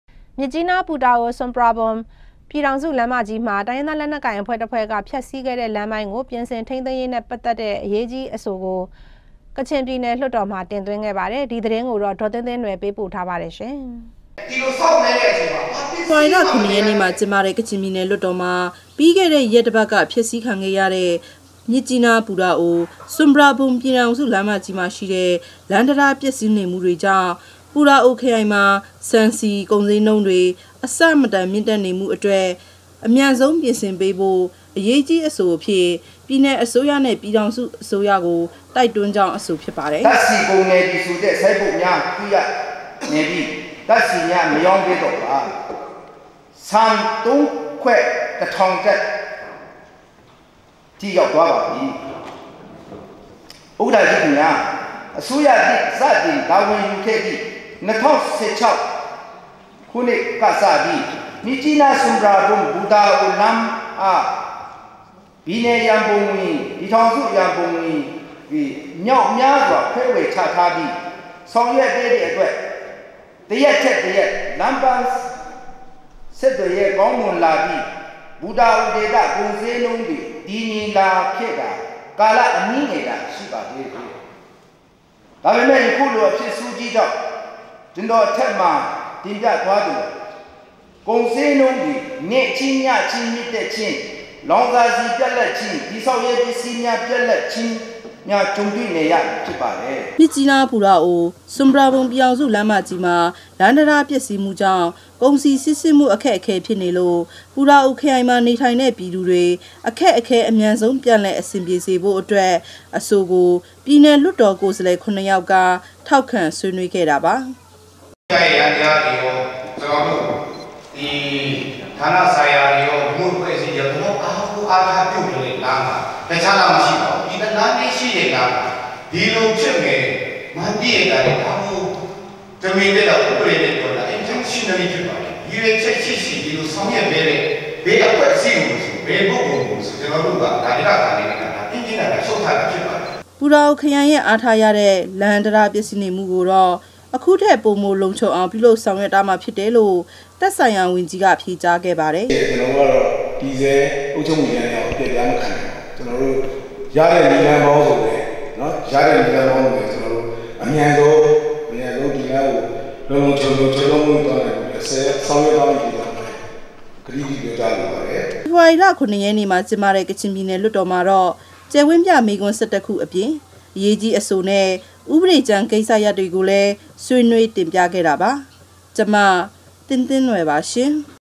DVB - ပူတာအုိမွ တုိင္းရင္းသားလက္နက္ကုိင္မ်ား ဖ်က္ဆီးခဲ့တဲ့လမ္းကုိ ျပန္လည္ျပင္ဆင္ေရး အဆုိတင္
DVB TV -  ျမစ္ၾကီးနား - ပူတာအို - ဆြမ္ပရာဘြမ္ ျပည္ေထာင္စုလမ္းမႀကီးမွာ တိုင္းရင္းသားလက္နက္ကိုင္အဖြဲ႔တစ္ဖြဲ႔က ဖ်က္စီးခဲ့တဲ့ လမ္းပိုင္းကို ျပင္ဆင္ထိန္းသိမ္းေရးနဲ႕ ပတ္သက္တဲ့အဆုိကို အေရးၾကီးအဆိုအျဖစ္  ကခ်င္ျပည္နယ္လႊတ္ေတာ္မွာ တင္သြြင္းခဲ့ပါတယ္။ ႔၊ဒီသတင္းကိုေတာ့ေဒၚသင္းသင္းႏြယ္ကသတင္းေပးပို႔ထားပါတယ္။